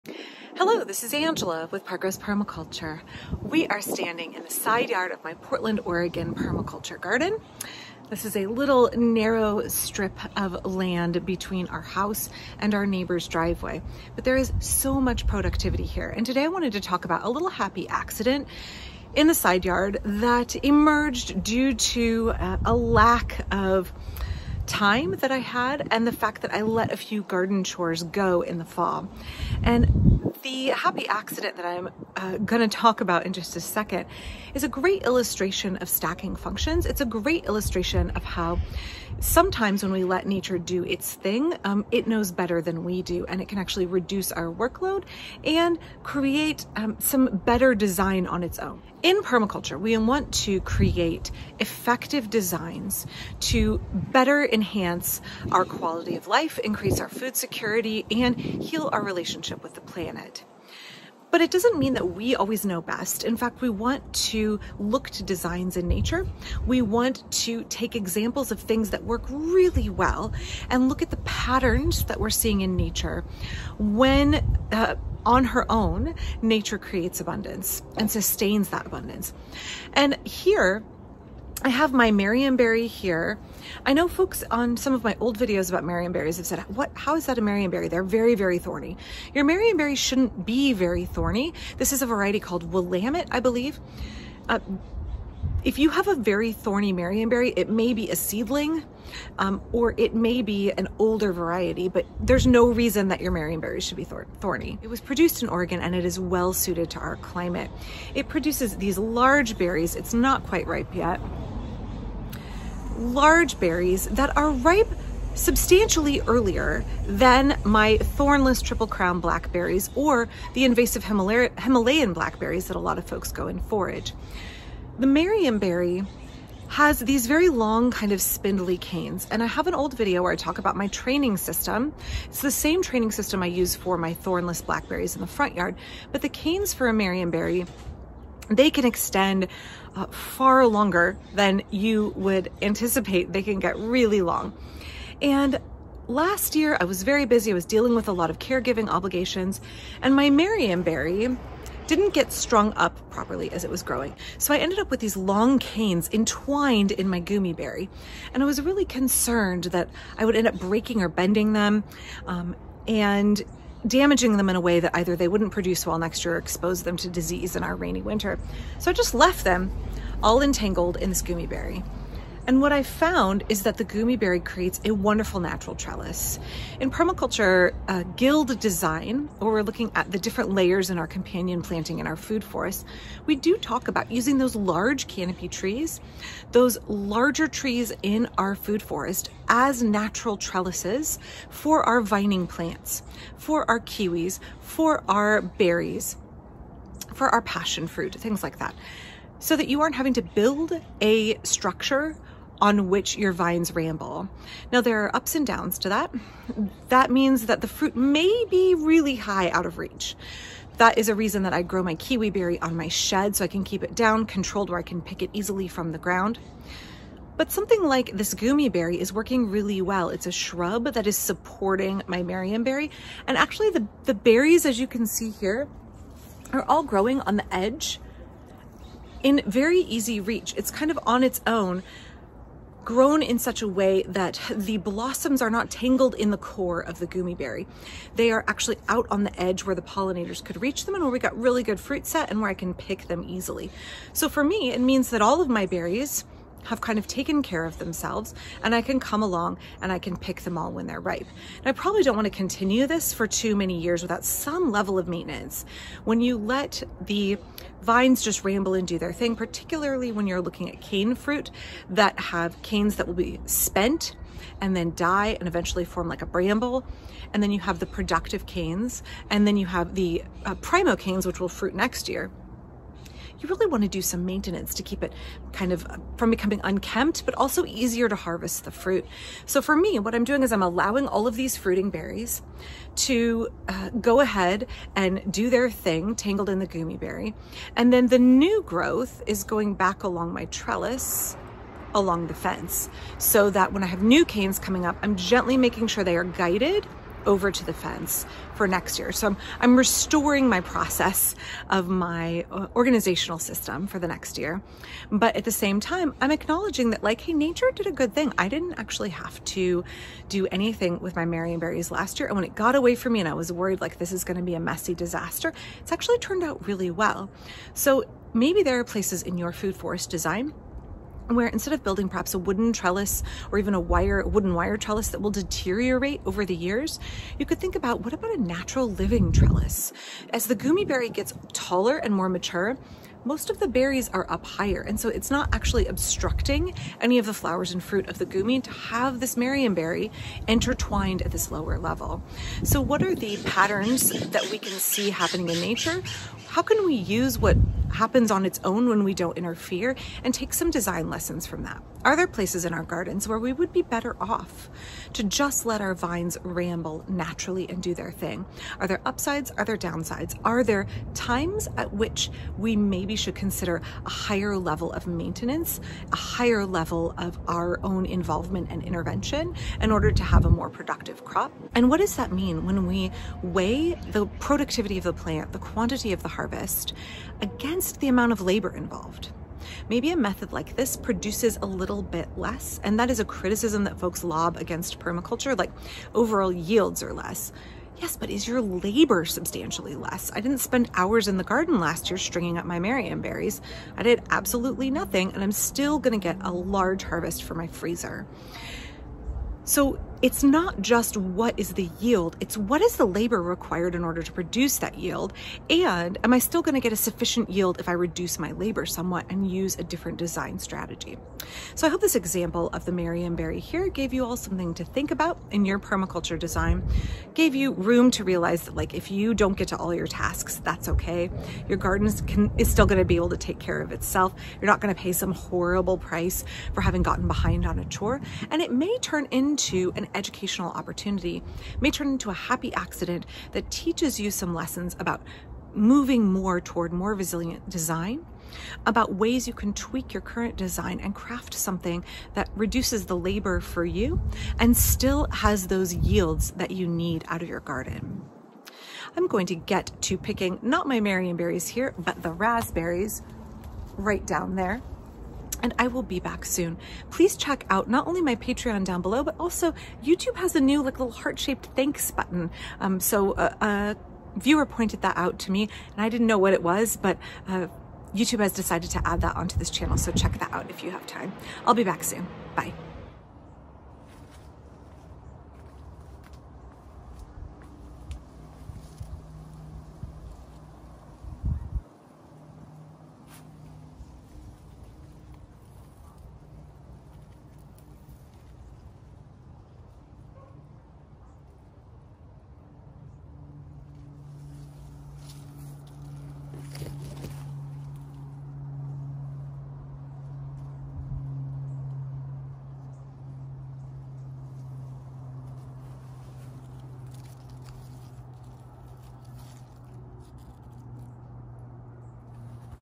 Mistakes & Neglect Can Teach Valuable Lessons about Garden Design : Wild Trellises, Berry Harvests
Permaculture is a design system that helps us turn mistakes and neglect into opportunity.  When we observe patterns created by our influence - or lack thereof! - in the garden, we can learn so much.   If we treat every situation  - including ones created by mistakes - as an opportunity to learn to design better, we are always pushing toward a more resilient garden, even when we don't always "keep up" with our chores and obligations.

Let's take a look at something that happened in my marionberry patch and ask, "Is Mother Nature doing better than I could have done?  What is working here and what needs help? Is this something I could learn from and tweak my garden design as a result?"

Thornless Marionberries:  "Willamette"